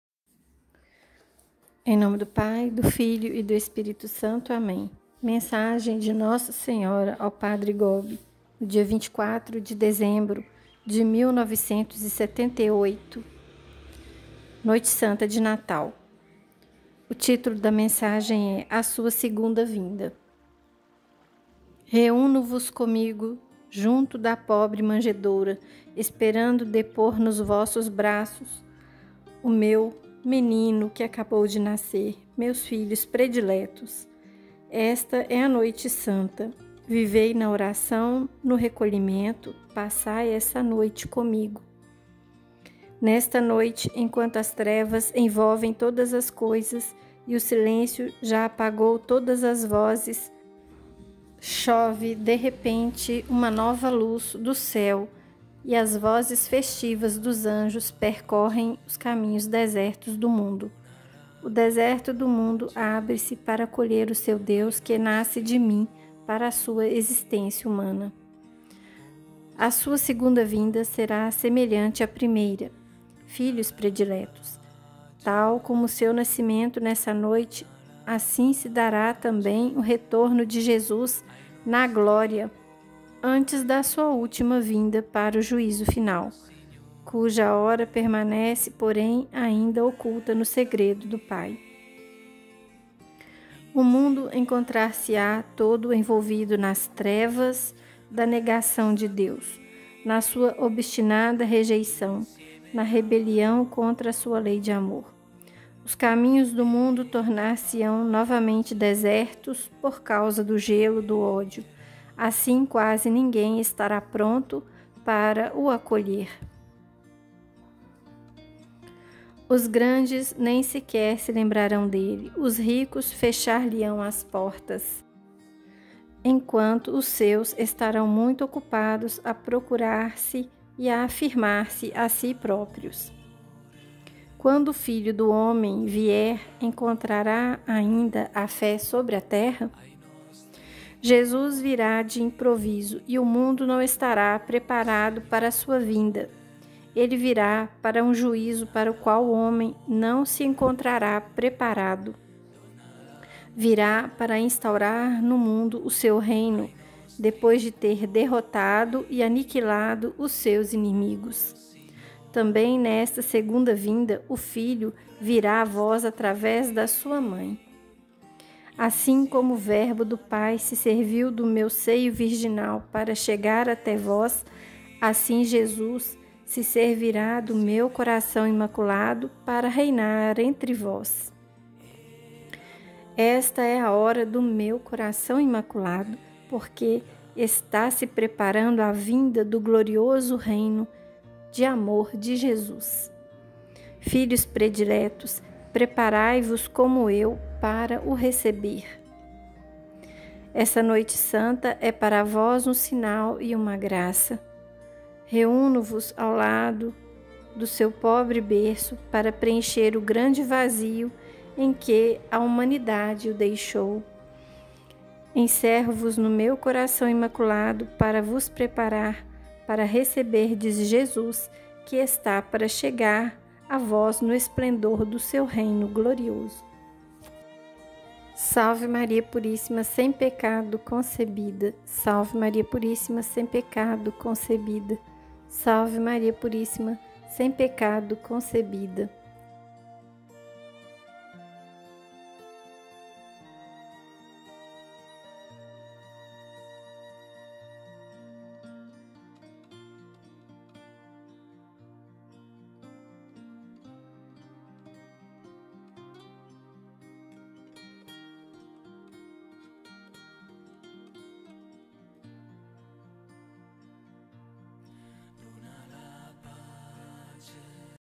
Mensagem de Nossa Senhora ao Padre Gobi, 24/12/1978, A Sua Segunda Vinda
adquira o livro azul
Aos Sacerdotes filhos prediletos de Nossa Senhora